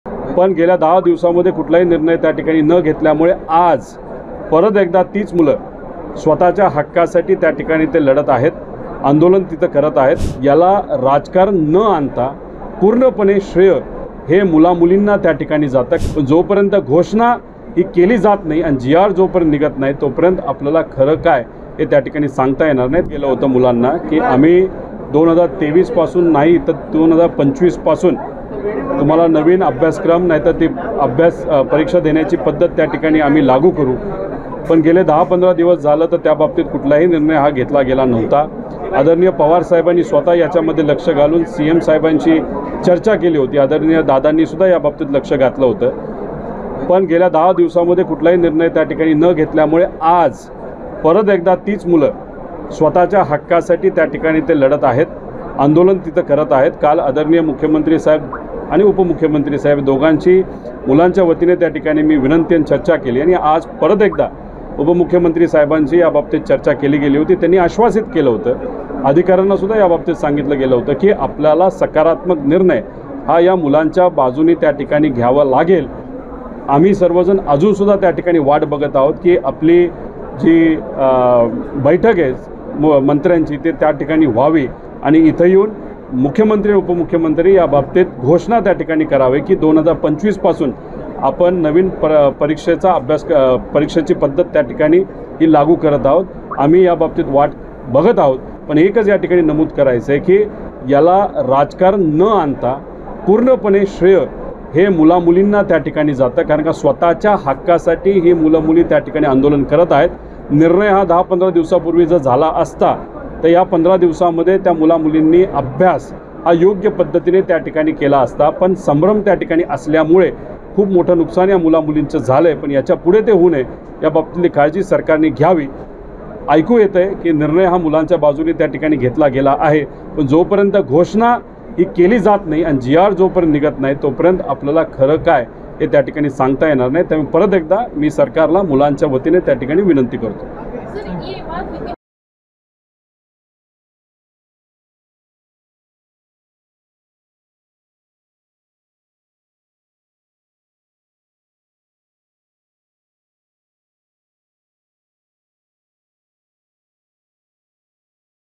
Rohit Pawar : श्रेय घेण्यावरून रोहित पवारांचा टोला... | LetsUpp Marathi
एमपीएसी परीक्षा पध्दतीत आयोगाकडून करण्यात आलेले बदल 2025 पासून लागू करावेत या मागणीसाठी पुण्यात विद्यार्थ्यांकडून आंदोलन करण्यात आले. या आंदोलनाला अखेर यश मिळालं आहे. मात्र विद्यार्थ्यी आंदोलनाचे श्रेय घेण्यावरून राष्ट्रवादीचे आमदार रोहित पवार यांनी प्रतिक्रिया दिली आहे.